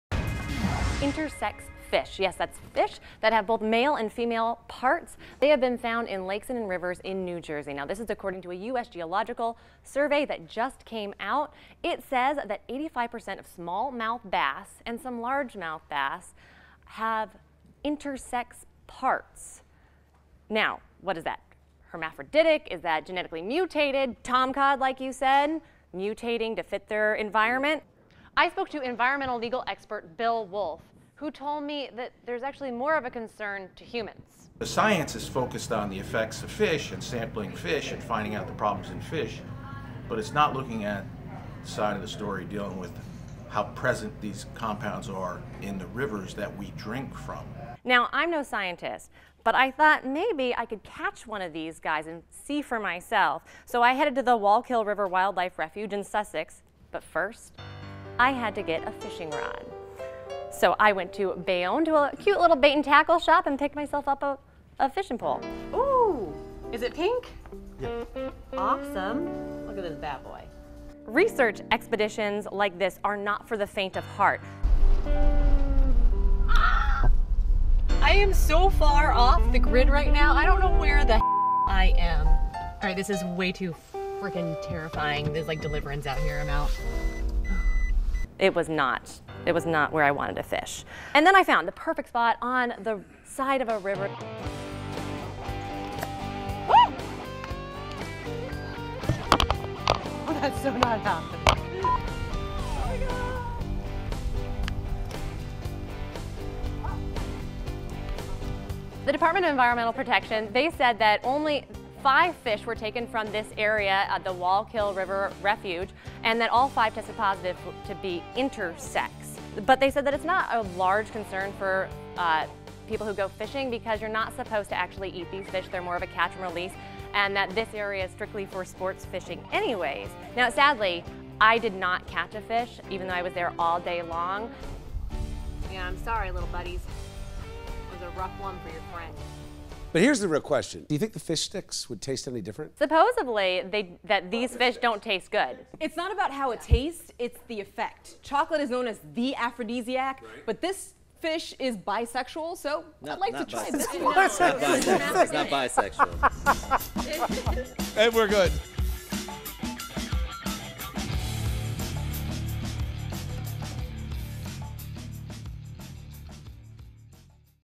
Something's Fishy In Sussex
Intersex fish, male fish with signs of female reproductive organs, have been found in rivers and lakes in New Jersey, according to a new US Geological Survey study of 19 National Wildlife Refuges throughout the Northeast.

The report showed that 85 percent of male smallmouth bass and 27 percent of male largemouth bass were found to have underdeveloped eggs in them.